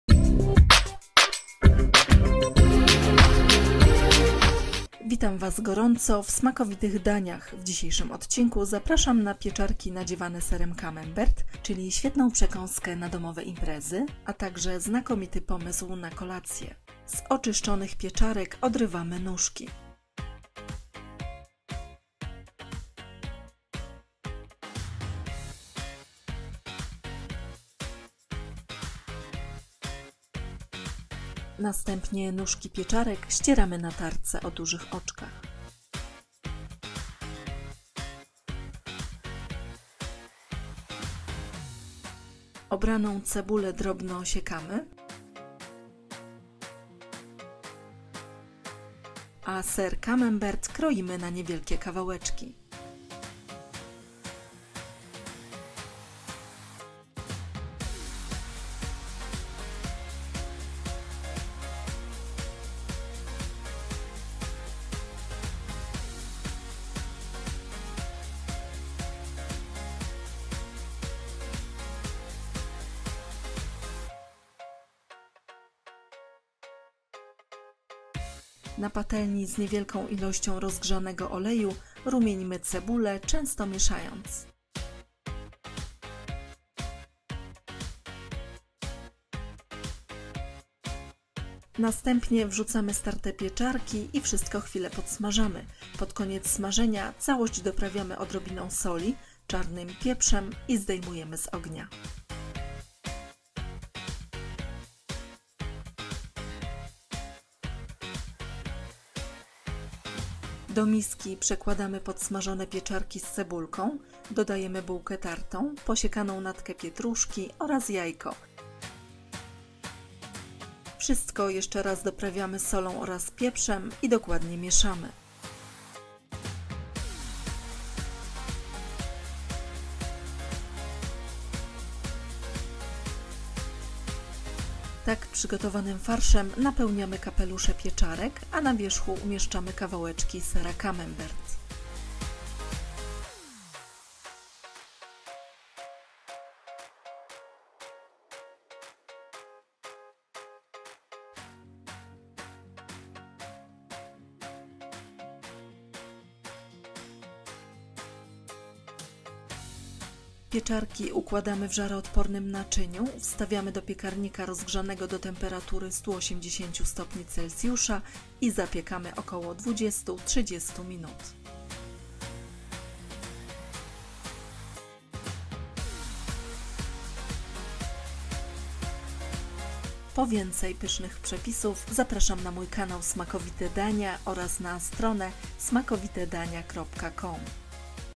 Pieczarki nadziewane serem camembert - Jak zrobić - Smakowite Dania
Pieczarki nadziewane serem camembert to pyszna przystawka na domowe imprezy, znakomity dodatek do obiadu lub ciekawy pomysł na ciepłą kolację. Takie pieczarki zachwycą smakiem każdego amatora sera camembert, można je również przygotować na grillu. Jak większość przystawek robi się je naprawdę szybko.

składniki na 10 sztuk

10 pieczarek

1 duża lub 2 małe cebule

1 jajko

120 g sera camembert

2 łyżki bułki tartej

kilka gałązek posiekanej natki pietruszki

czarny pieprz

sól